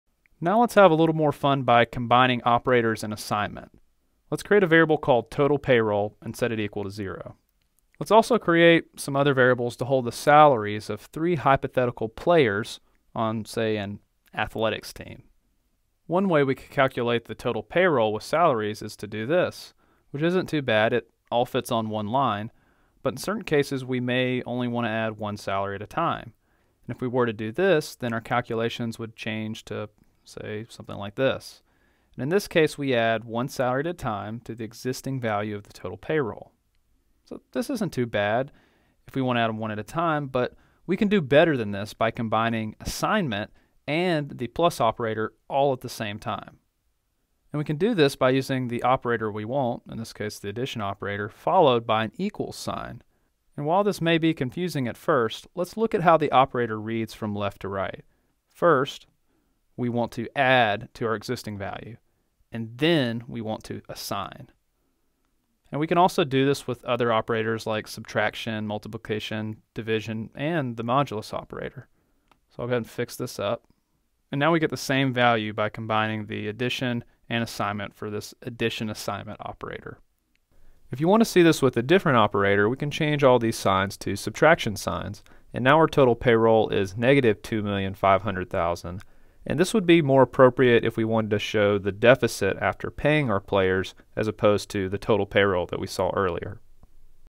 The Gotchas of Operators Part 2
Examples of Swift operators using Playground.